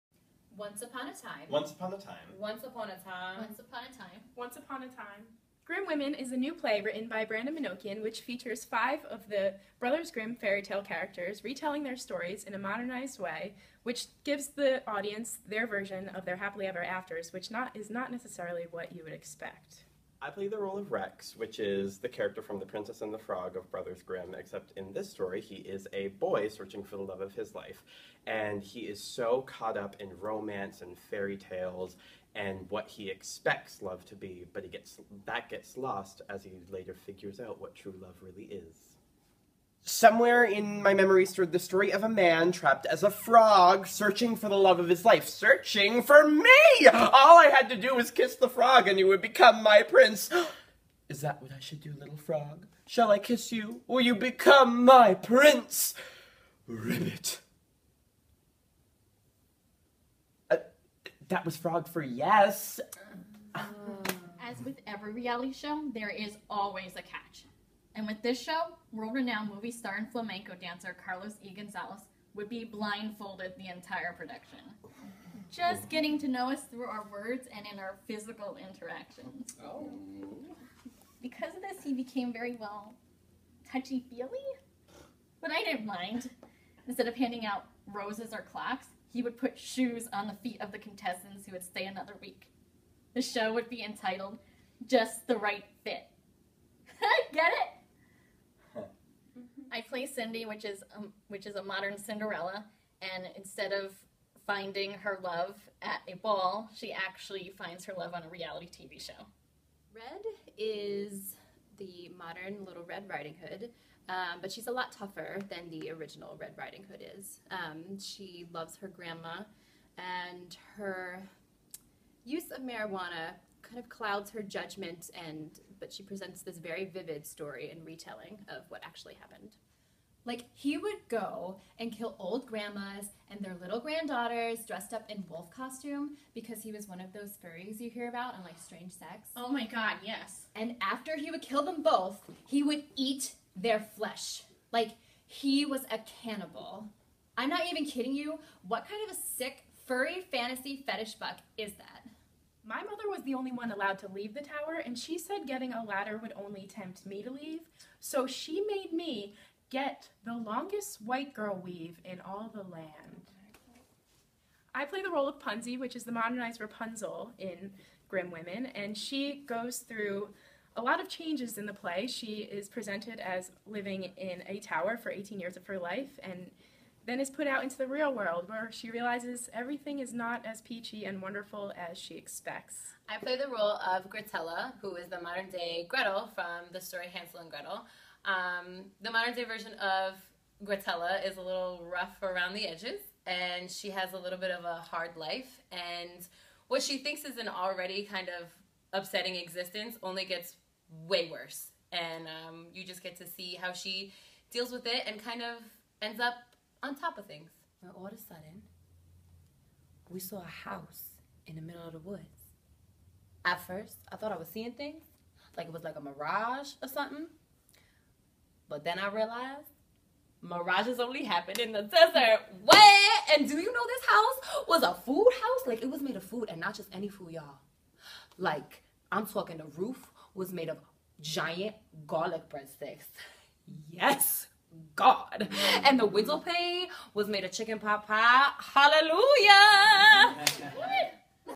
Grimm Women May 2014
Grimm Women

Friday, May 23 9:30pm
Saturday, May 24 8:00pm

The West End Cabaret Theatre
955 West End Ave. @107th and Broadway
NYC

$5 cover for students and artists
$15 cover for adults
$15 drink/food minimum per person

Written by Brandon Monokian
Directed by Megan Greener

Starring:
Ashley Biel
Daria Feneis
Caitlin Gutches
Andrew Pace